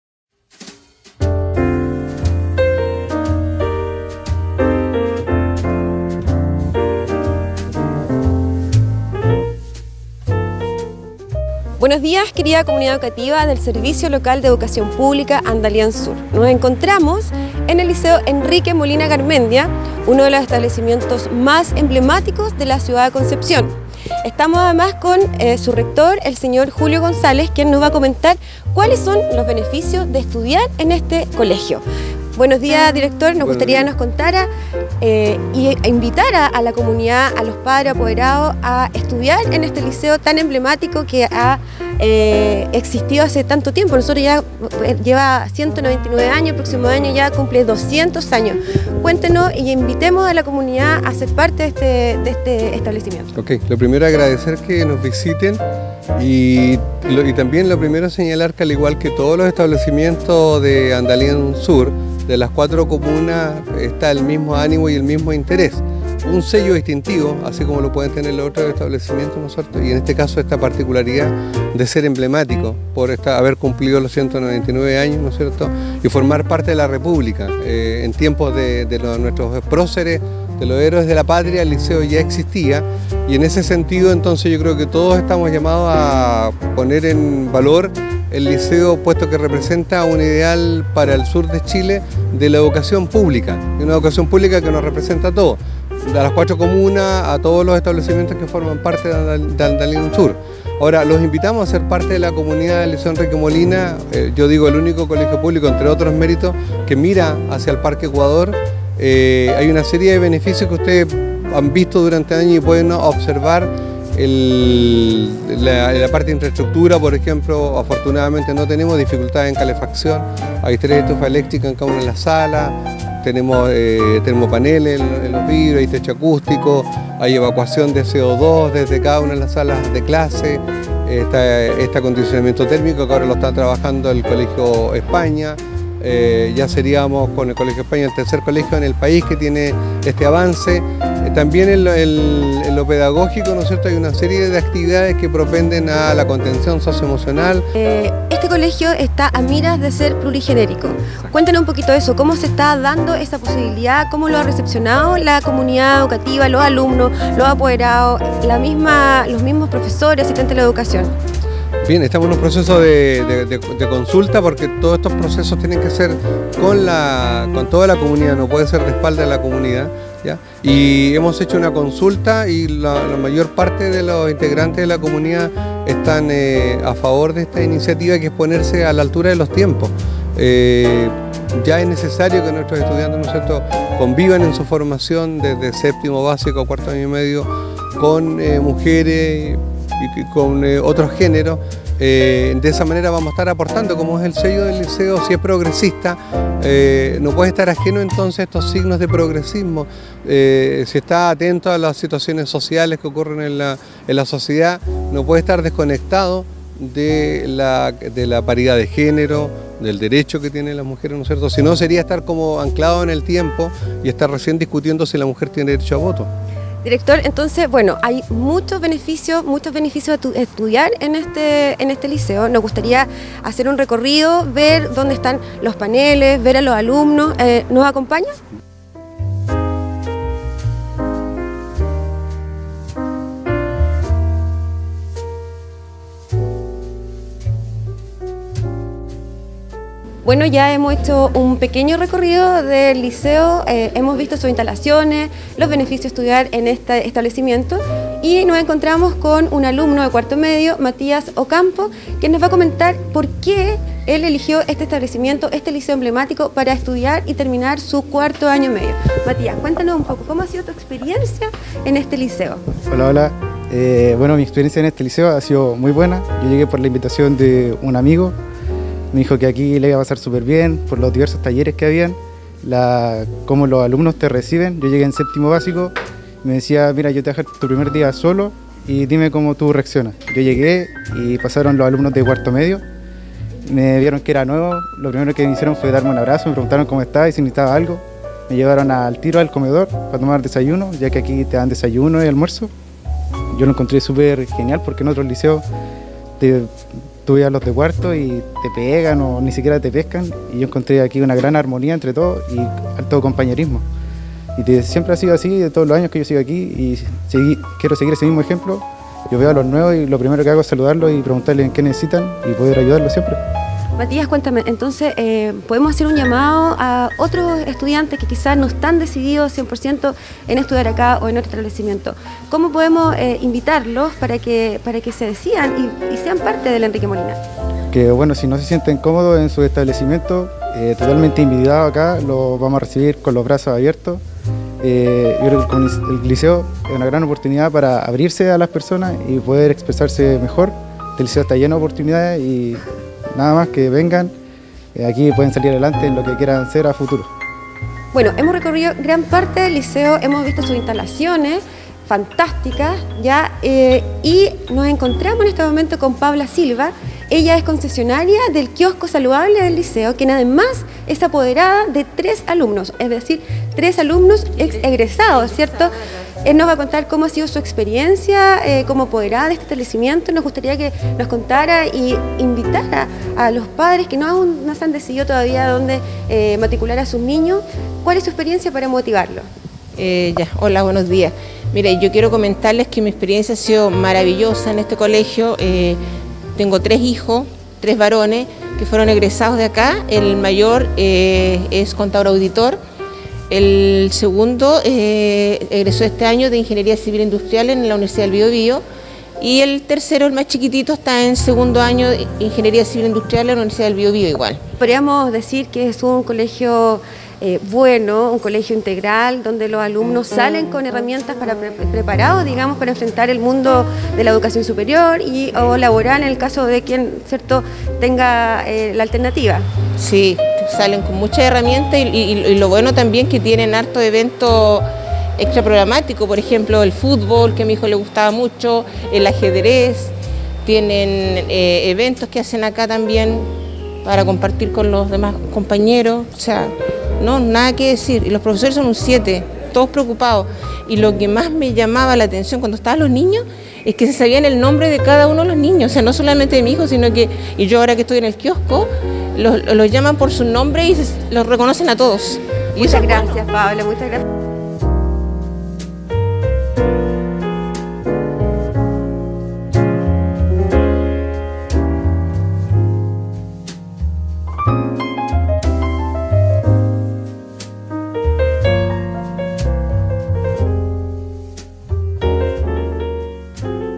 POR QUÉ ESTUDIAR EN MI COLEGIO
👉Los invitamos a ver esta nota 🎥 que cuenta los beneficios de estudiar en los establecimientos del Servicio Local de Educación Pública Andalién Sur. En esta oportunidad visitamos el Liceo Enrique Molina Garmendia. Su rector, apoderados y alumnos nos cuentan su experiencia e invitan a la comunidad a ser parte de la familia EMG ✨💫